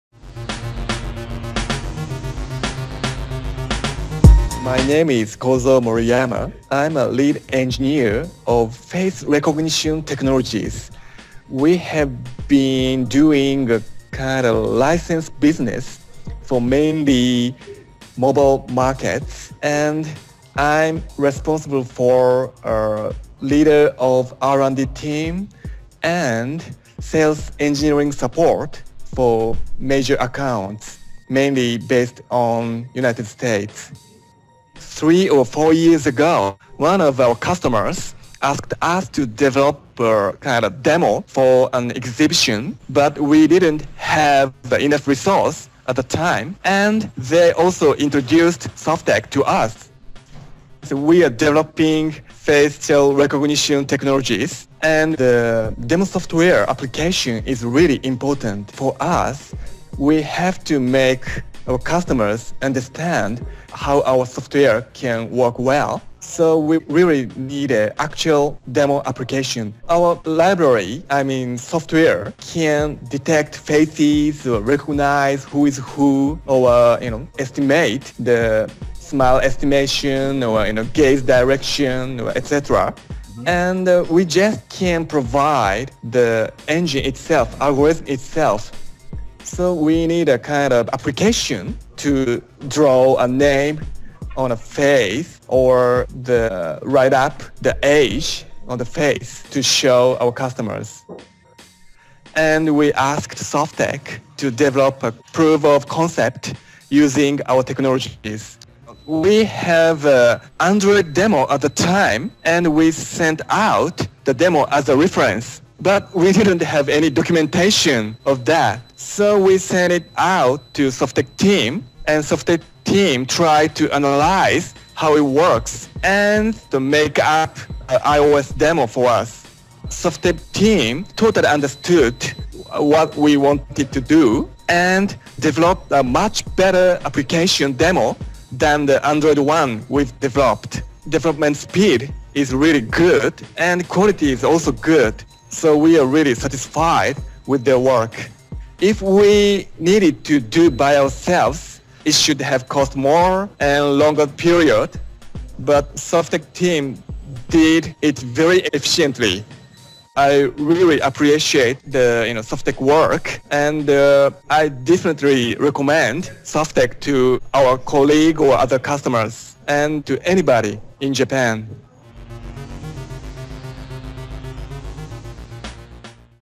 OMRON's Lead Engineer on face recognition demo app by Softeq
Mr. Kozo Moriyama speaks about the proof-of-concept iOS app that Softeq developed for OMRON's R&D department to demo their innovative face recognition software. He points out the analytical skill of the Softeq team, high quality and speed of work. Another client is highly satisfied and provides a great reference. Thanks so much, Kozo!

0:34 How did you learn about Softeq?
0:50 Project in focus
1:11 Main features of the product
1:49 What services did Softeq provide to you?
2:09 Are you satisfied with the results of cooperation?
2:36 Would you recommend Softeq to another company?